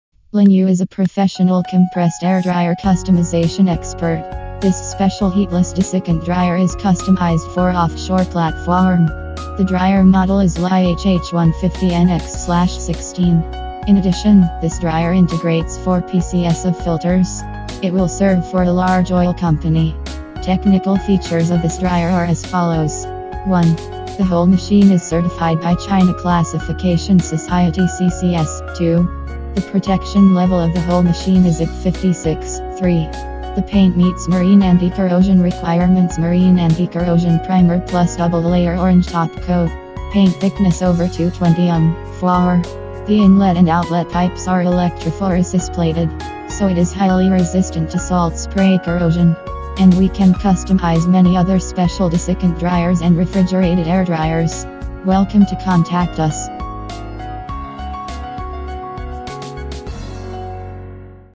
IP56 Marine Anti corrosion Custom Heatless Desiccant Air Dryer
Lingyu is a professional compressed air dryer customization expert. This special heatless desiccant dryer is customized for offshore platform. The dryer model is LY-HH150NX/16. In addition, this dryer integrates 4pcs of filters. It will serve for a large oil company.

Technical features of this dryer are as follows:
1. The whole machine is certified by China Classification Society (CCS);
2. The protection level of the whole machine is IP56;
3. The paint meets marine anti-corrosion requirements (marine anti-corrosion primer + double-layer orange topcoat). Paint thickness over 220μm;
4. The inlet and outlet pipes are electrophoresis-plated. So it is highly resistant to salt spray corrosion.

And we can customize many other special desiccant dryers and refrigerated air dryers.